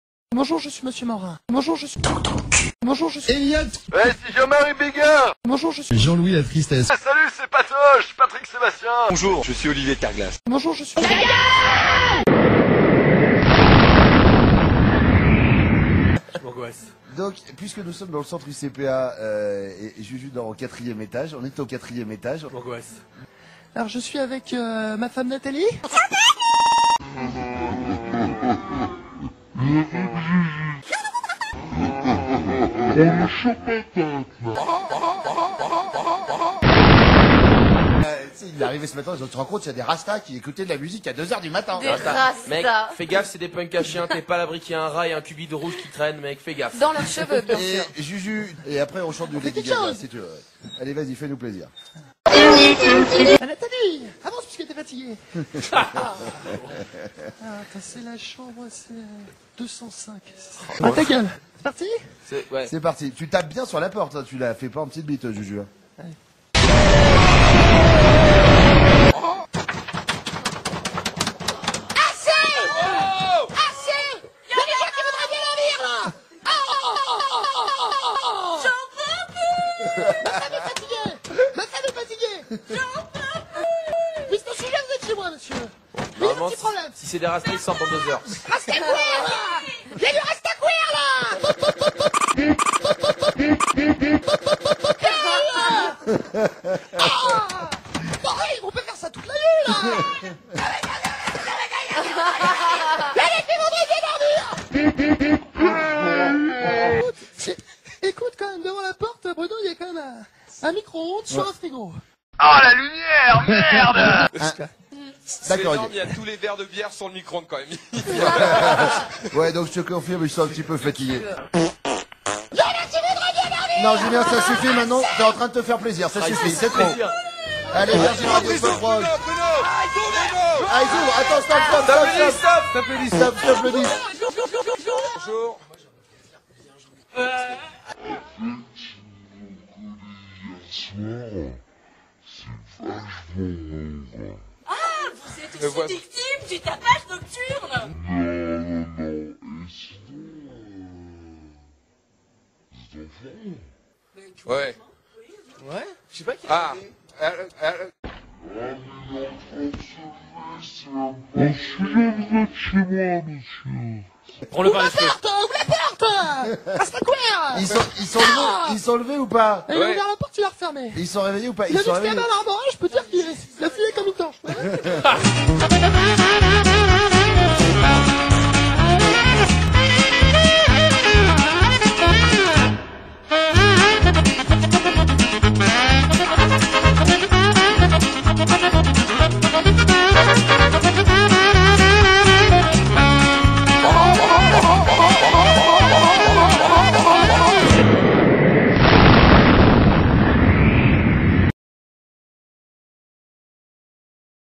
[Youtube Poop French] Bruno Guillon à Tignes - Julien réveille des fêtards
Je pense que c'est mon meilleur Youtube Poop : celui où Julien va réveiller ses voisins fêtards à... 6H DU MAT !
C'était déjà drôle dans la version originale calquée sur les Bronzés, mais là ne prévoyez pas de nourriture ou d'eau en regardant la vidéo !